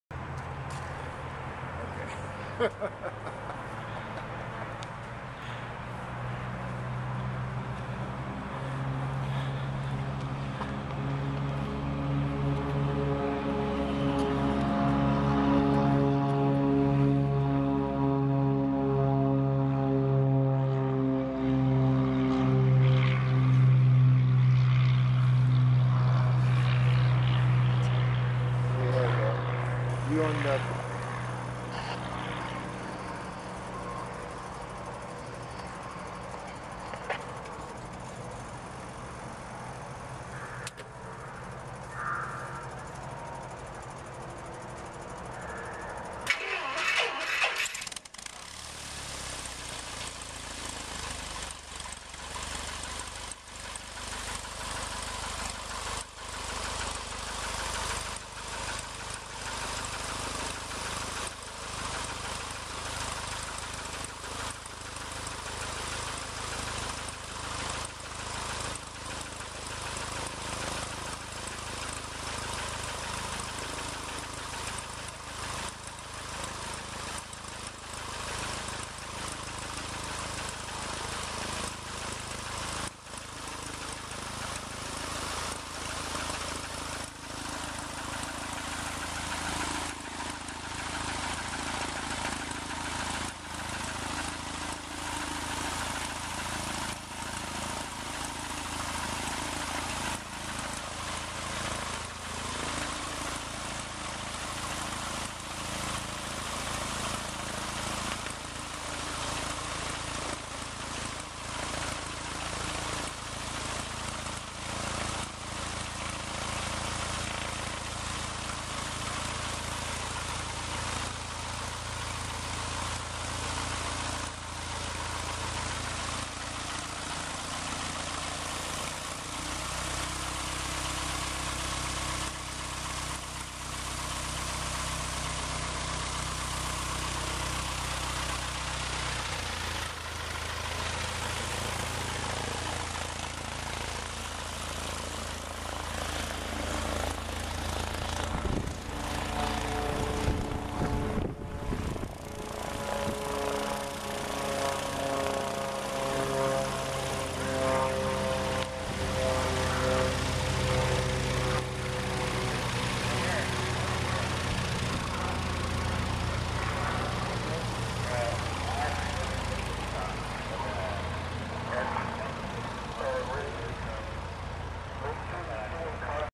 Airplane With 1.2GHZ ATV Onboard 11/05/11
This is Eric's (N6VYT) airplane used for the test.  This is just a startup and warmup video.  The plane is called "Huff-N-Puff"  This video footage was recorded at the Visalia Airport.